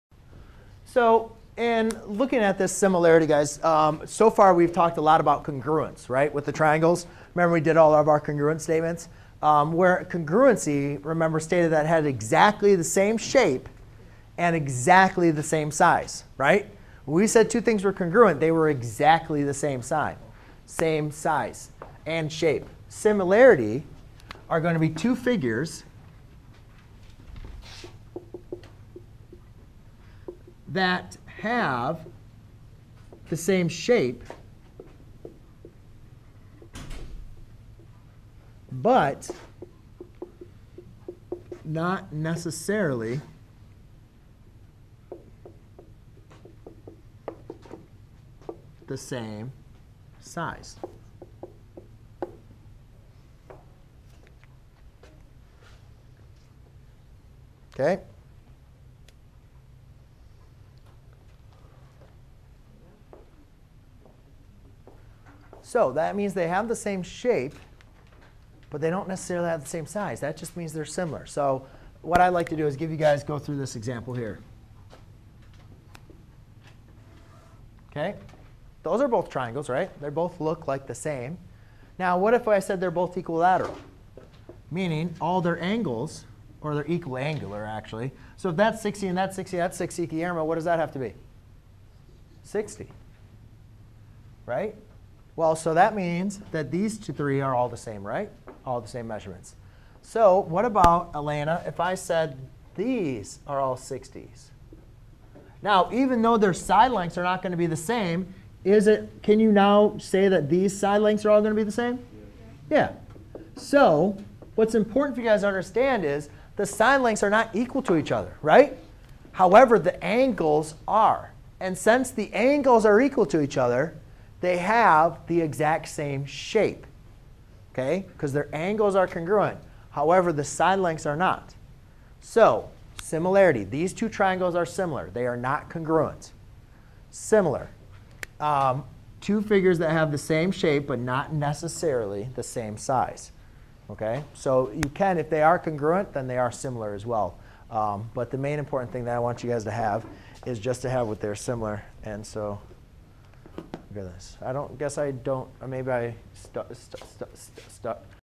What is similarity
👉 Learn how to solve with similar triangles. Two triangles are said to be similar if the corresponding angles are congruent (equal). Note that two triangles are similar does not imply that the length of the sides are equal but the sides are proportional. Knowledge of the length of the sides or the proportion of the side lengths of one of the similar triangles and one side length of the other triangle enables us to find the side lengths of the other sides of the other triangle.